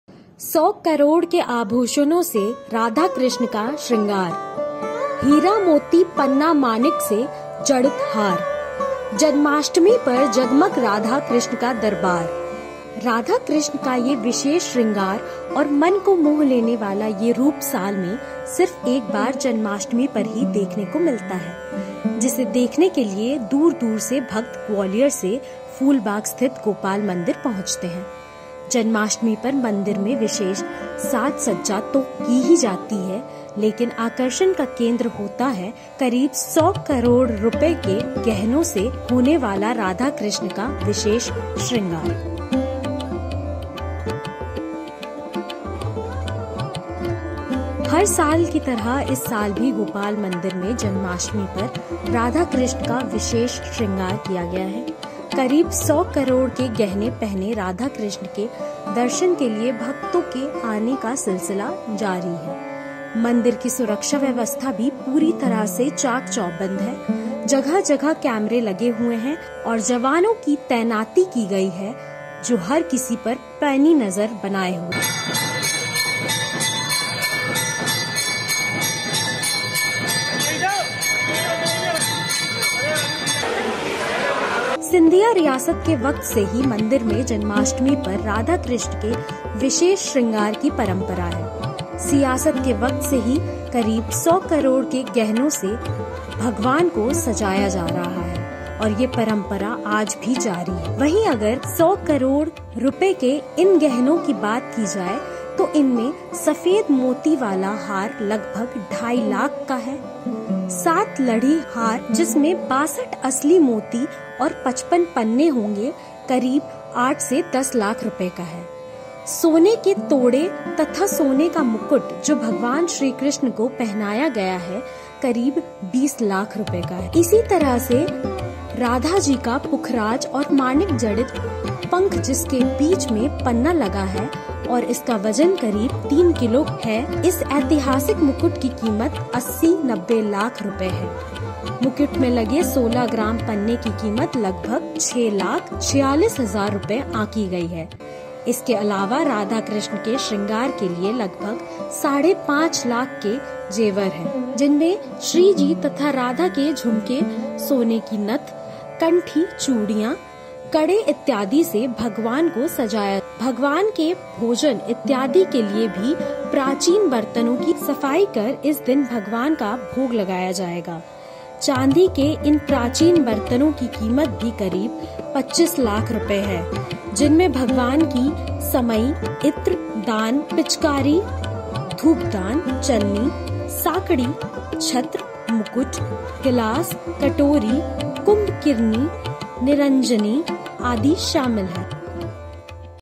देश का एकलौता मंदिर जहां 100 करोड़ के गहनों से हुआ राधा-कृष्ण का श्रृंगार | Gopal Mandir | Gwalior
फूलबाग स्थित गोपाल मंदिर में गुरुवार को जन्माष्टमी पर्व पर श्रीराधा-कृष्ण का करोड़ों रुपए कीमत के आभूषण से श्रृंगार किया गया। हीरे-मोती, पन्ना-माणिक जैसे रत्नजडि़त सोने-चांदी के प्राचीन आभूषण देखने के लिए अभी से श्रद्धालुओं का तांता लग गया।